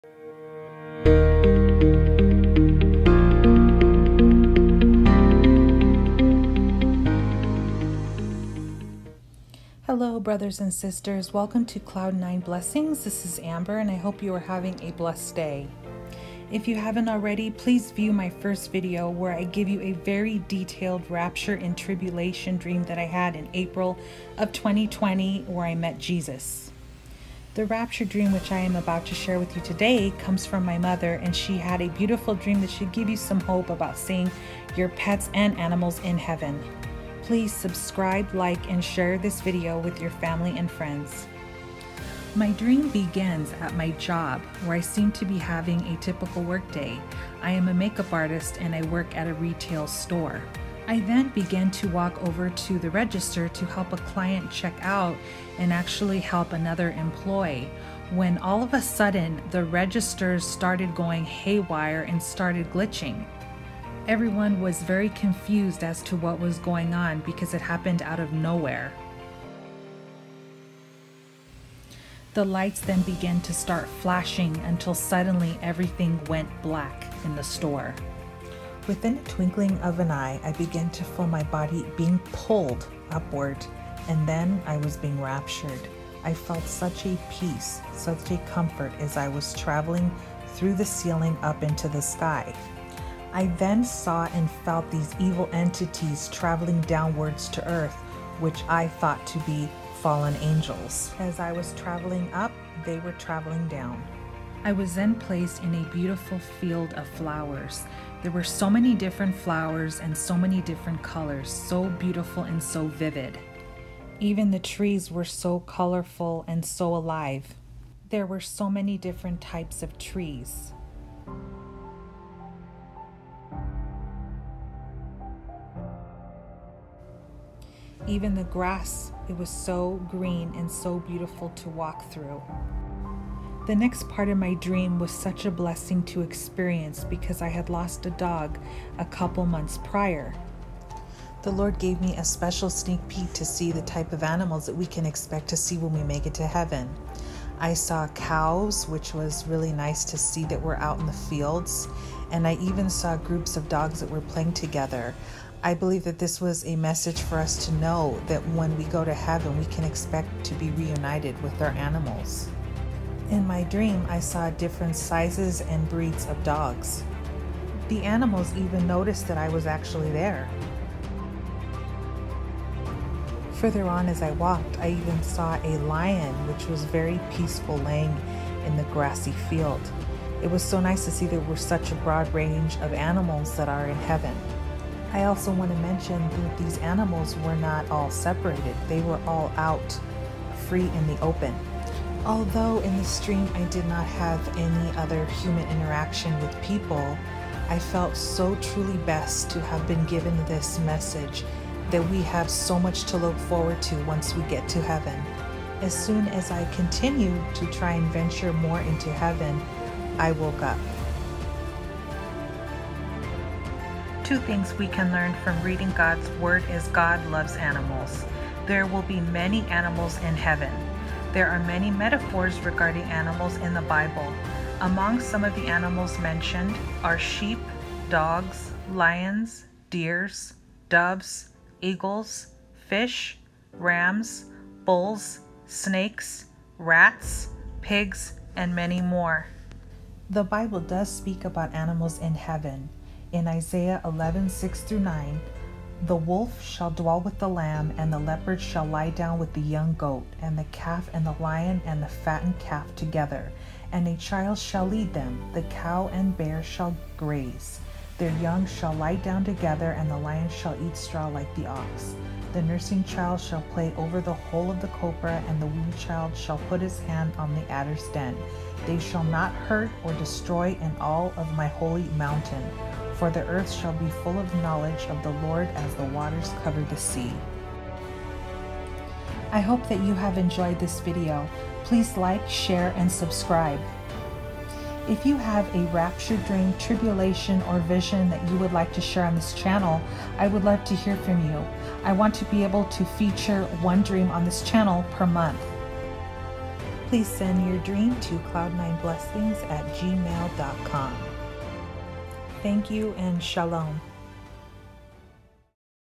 All ANIMALS DO GO TO HEAVEN! RAPTURE VISION 2020!
A beautiful rapture dream that should give everyone hope of seeing there pets and animals when they get to heaven. God gives my mother a sneak peak of the wonderful things he has in store for us!

If you feel lead to, Please Support Cloud 9 Blessings by making a donation! That would be greatly appreciated! Thank you and god bless!